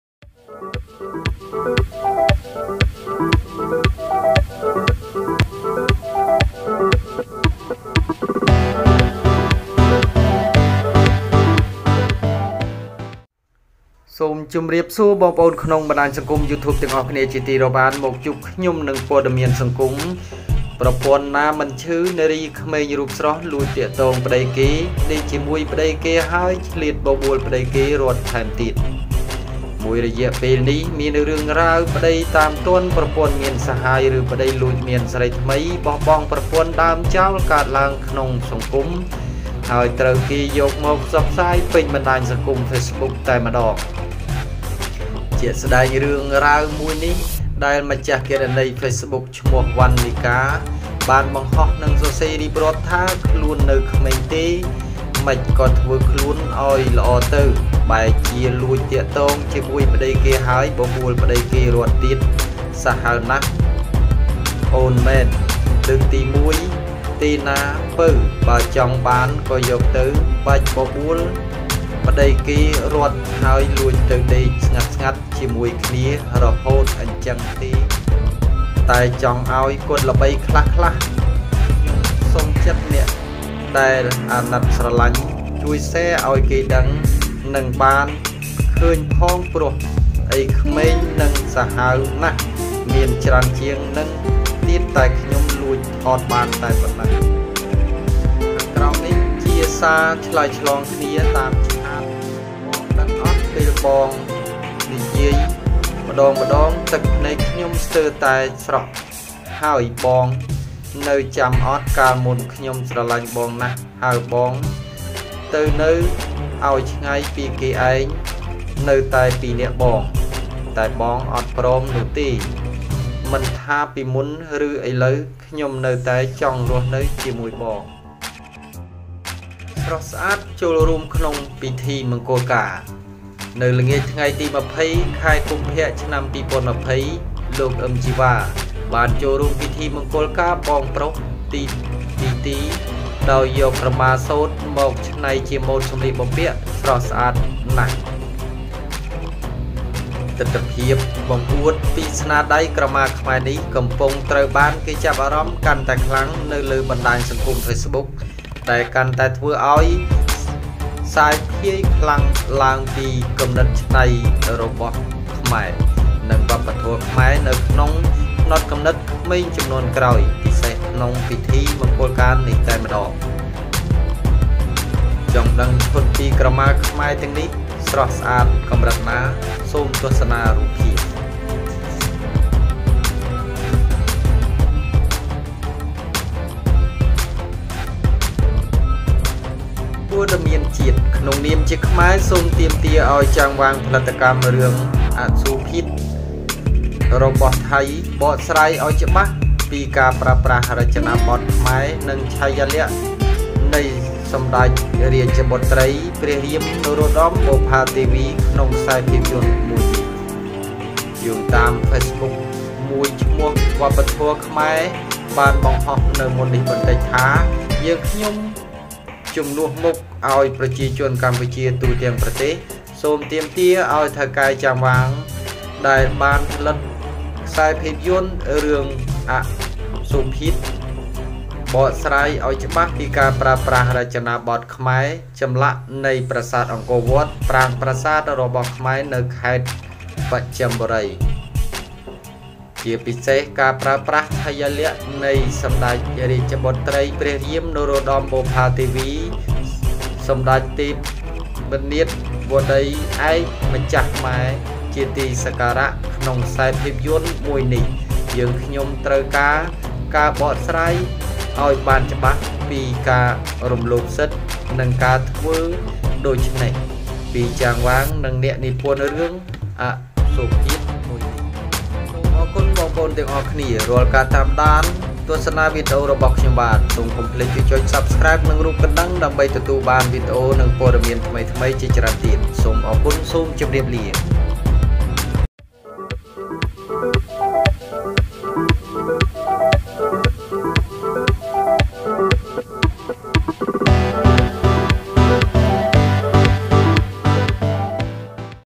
កប់បាត់!! ស្រីណាមិនឈឺនារីរូបស្រស់ទាក់ទងប្ដីគេ Yan Sun
កប់បាត់!! ស្រីណាមិនឈឺនារីរូបស្រស់ទាក់ទងប្ដីគេហើយ នៅចង់ប្ដីគេរត់ទៀត

ព័ត៌មានថ្មីៗ
ព័ត៌មានទាន់ហេតុការណ៍ប្រចាំថ្ងៃ
ព័ត៌មានប្លែកៗនិងកំសាន្ត